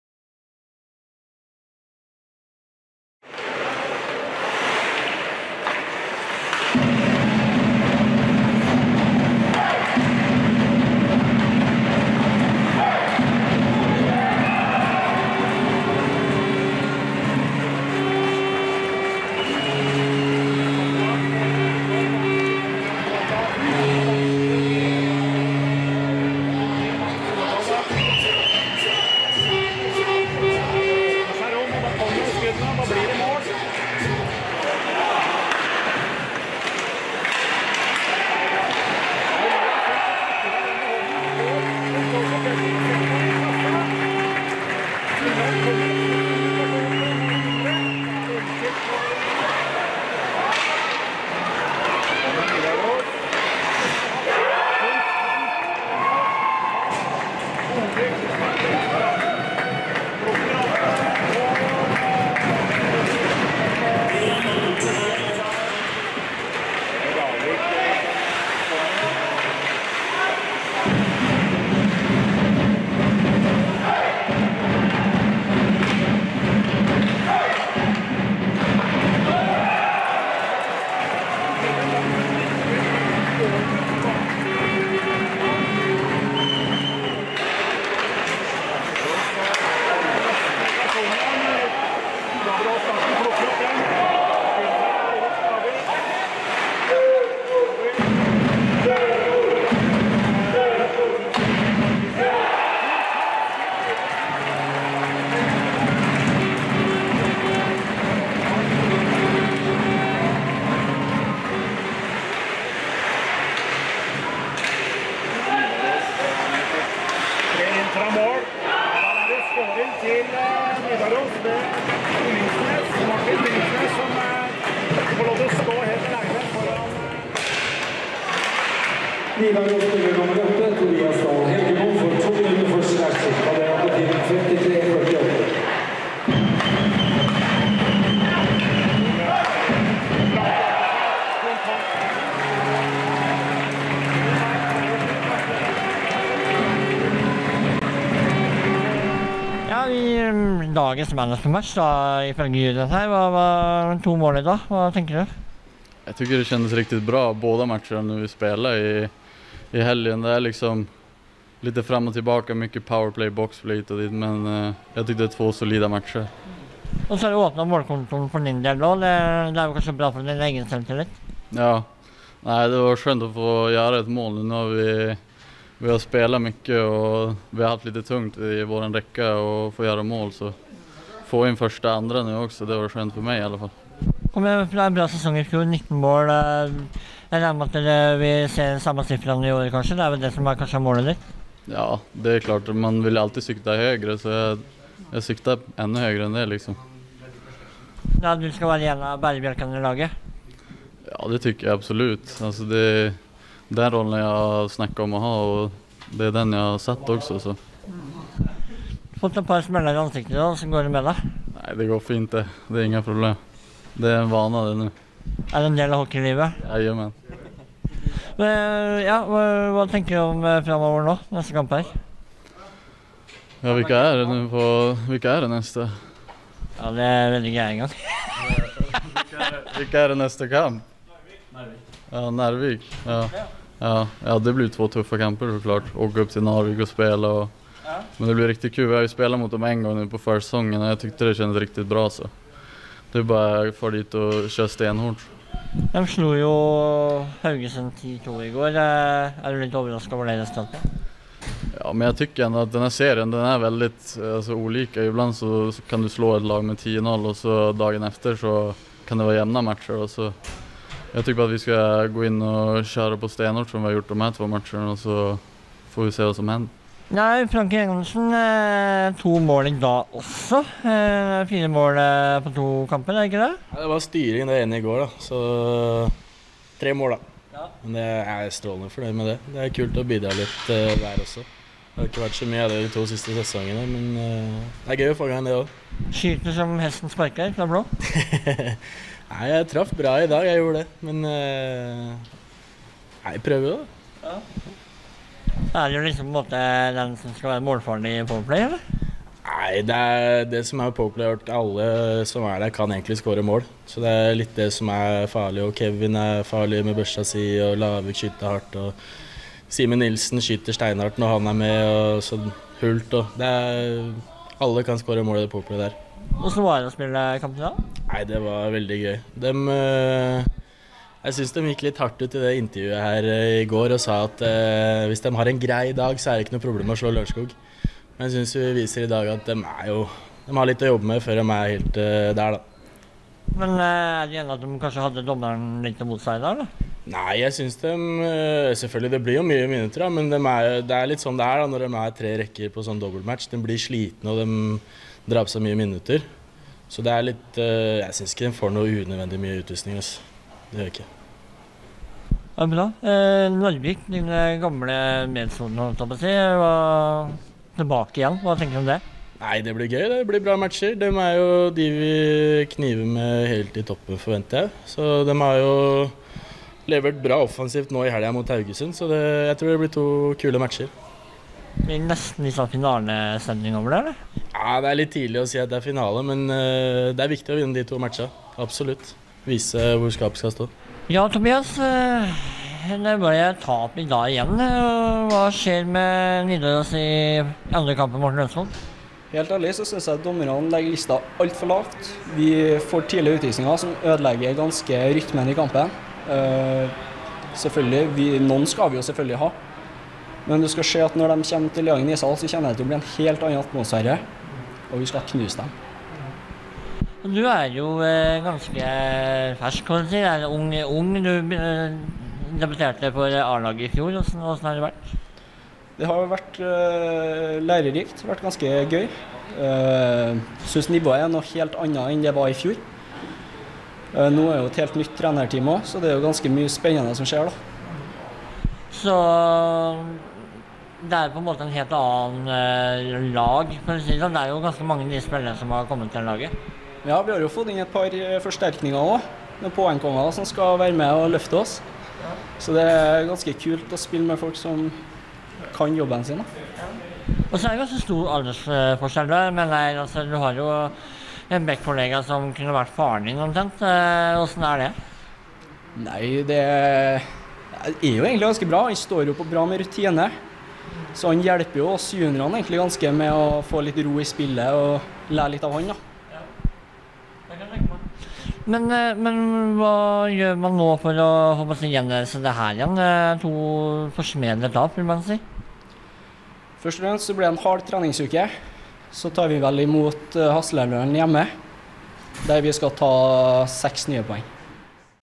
Lørenskog - Nidaros 5-2 (1. div.)(031021)
Høydepunkter og intervju med Elias Hult og Lars Erik Francke-Enersen hos Lørenskog og Tobias Helgemo hos Nidaros.